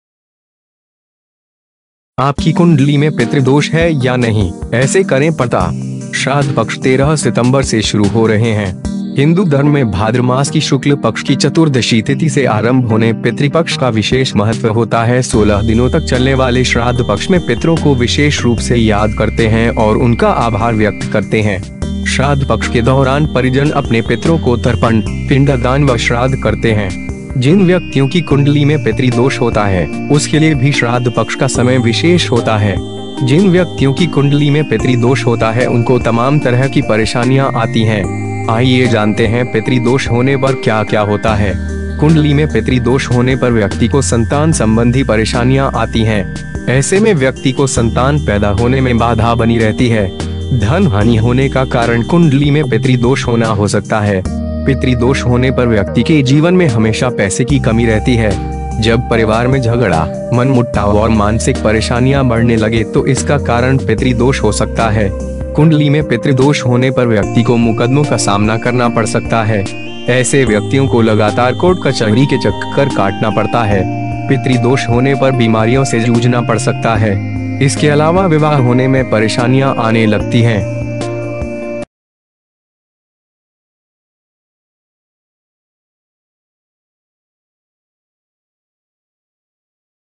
आपकी कुंडली में पितृदोष है या नहीं, ऐसे करें पता
श्राद्ध पक्ष 13 सितंबर से शुरू हो रहे हैं। हिन्दू धर्म में भाद्र मास की शुक्ल पक्ष की चतुर्दशी तिथि से आरम्भ होने पितृपक्ष का विशेष महत्व होता है। 16 दिनों तक चलने वाले श्राद्ध पक्ष में पितरों को विशेष रूप से याद करते हैं और उनका आभार व्यक्त करते हैं।  श्राद्ध पक्ष के दौरान परिजन अपने पितरों को तर्पण, पिण्डदान व श्राद्ध करते हैं। जिन व्यक्तियों की कुंडली में पितृ दोष होता है, उसके लिए भी श्राद्ध पक्ष का समय विशेष होता है। जिन व्यक्तियों की कुंडली में पितृ दोष होता है उनको तमाम तरह की परेशानियां आती हैं। आइए जानते हैं पितृ दोष होने पर क्या-क्या होता है।

 कुंडली में पितृ दोष होने पर व्यक्ति को संतान संबंधी परेशानियां आती हैं। ऐसे में व्यक्ति को संतान पैदा होने में बाधा बनी रहती है।

धन हानि होने का कारण कुंडली में पितृ दोष होना हो सकता है। पितृ दोष होने पर व्यक्ति के जीवन में हमेशा पैसे की कमी रहती है।

जब परिवार में झगड़ा, मनमुटाव और मानसिक परेशानियां बढ़ने लगे तो इसका कारण पितृ दोष हो सकता है।

कुंडली में पितृदोष होने पर व्यक्ति को मुकदमों का सामना करना पड़ सकता है। ऐसे व्यक्तियों को लगातार कोर्ट-कचहरी के चक्कर काटना पड़ता है।

पितृ दोष होने पर बीमारियों से जूझना पड़ सकता है। इसके अलावा विवाह होने में परेशानियां आने लगती हैं।